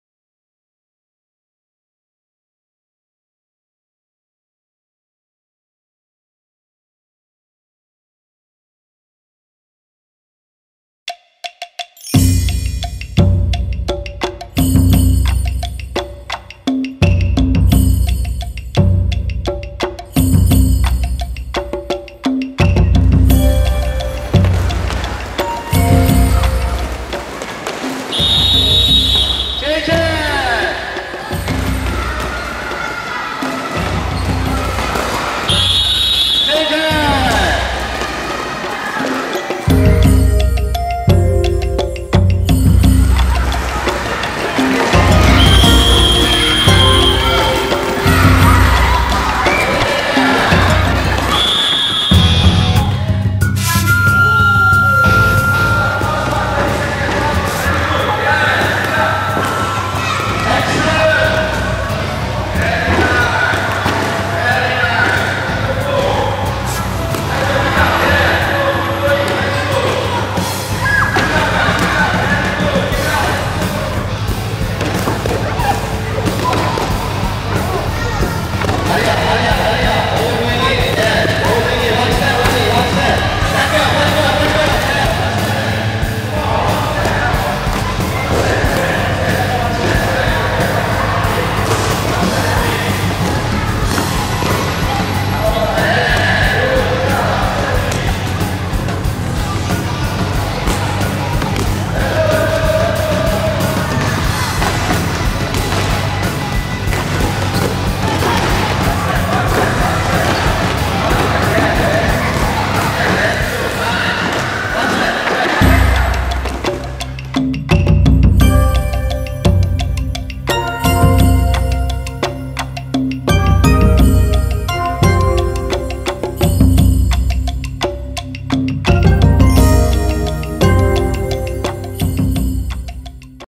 Fuchun Sec Sch SEP 2010
Sports Chanbara SEP at Fuchun Secondary School by Samurai Sports Singapore, 2010